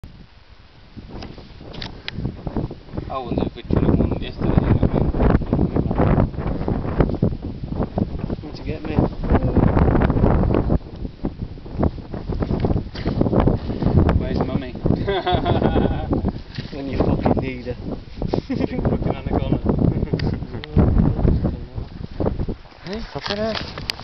16 A real snake.AVI
Whilst on the last leg of the Lyke Wake Walk an Adder snake decided to say hello.